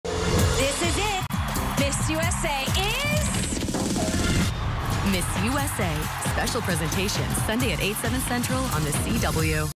Watch Miss USA on CW39 Houston Sunday August 4th at 7 p.m (CST)
CW39's Sharron Melton will be at the Miss USA and Miss Teen USA this weekend. Check out EXCLUSIVE behind the scenes looks on CW39 social media and online at CW39.com.